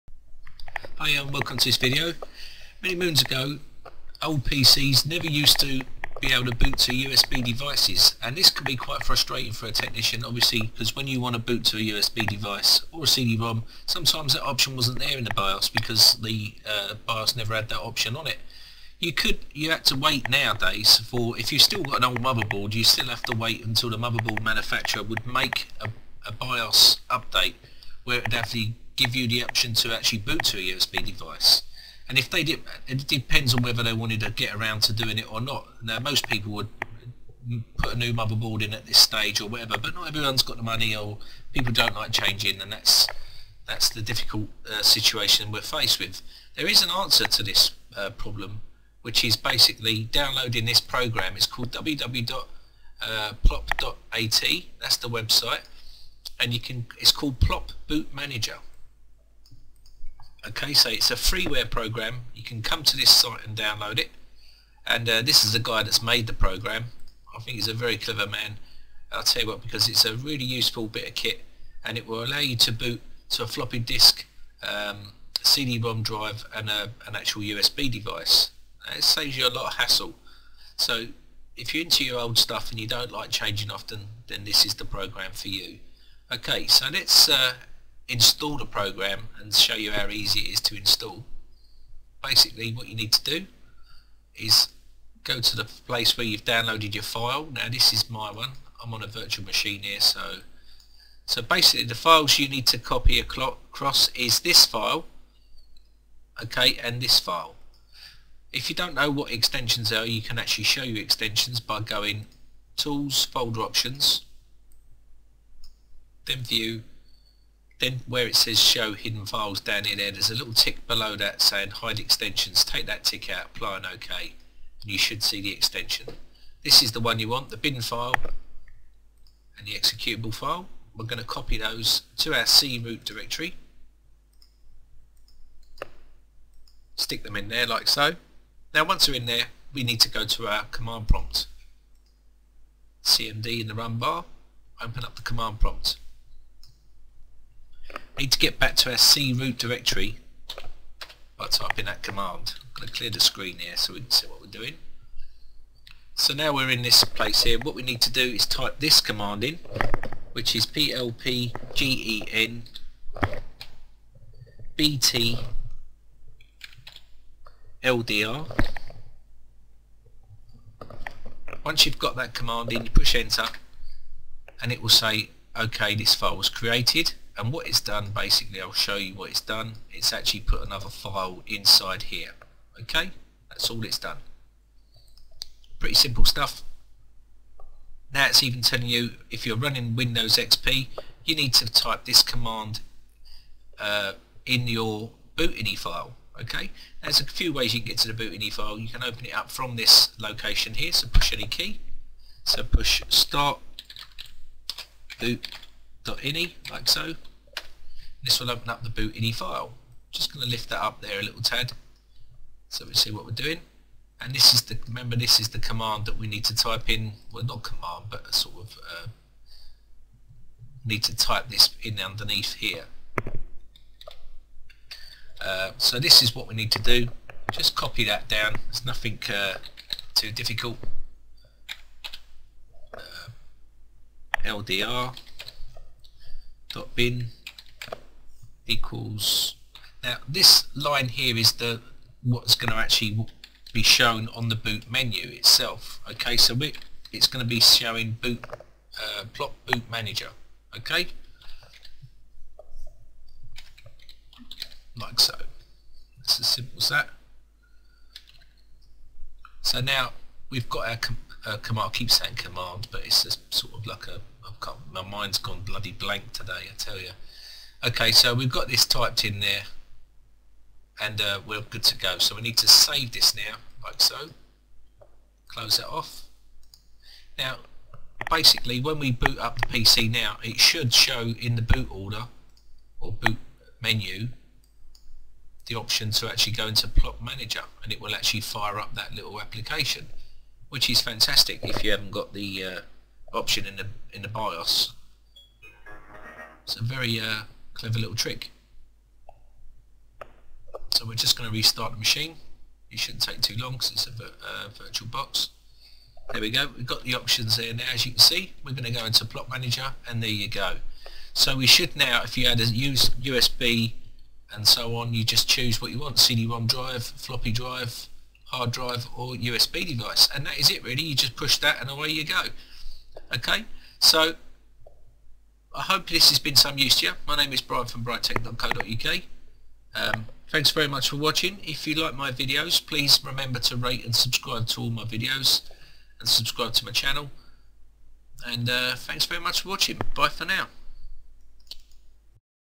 Boot Computer From USB or CDROM Using PLoP Even If BIOS is Not Supported by Britec
Boot Computer From USB or CDROM Using PLoP Even If BIOS is Not Supported

The PLoP Boot Manager is a small program to boot different operating systems. You can boot the operating systems from harddisk, floppy, CD/DVD or from USB.

You can start the boot manager from floppy, CD, network and there are many more ways to start the boot manager.

You can install the boot manager on your harddisk. There is no extra partition required for the boot manager.

The PLoP Boot Manager is written by Elmar Hanlhofer.

The current version is plpbt-5.0.3.7z